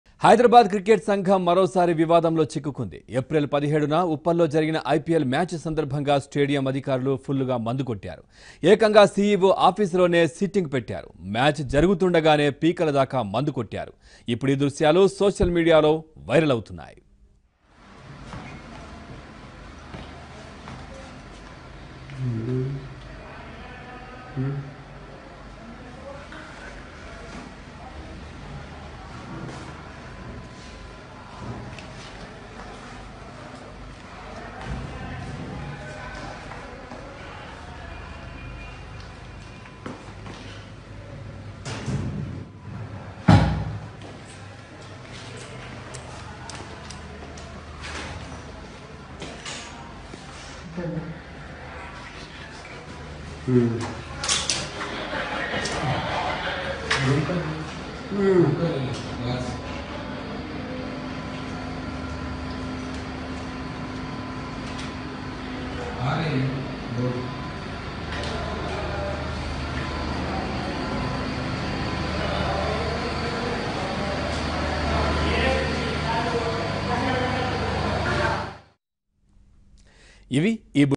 Liquor Party at Hyderabad Cricket Association CEO Office in Uppal Stadium | ABN Telugu
Liquor party at Hyderabad cricket association CEO office in Uppal stadium. They have organized liquor party in the office at a time when the IPL match was going on April 17th. The visuals of this party are trending across social media platforms.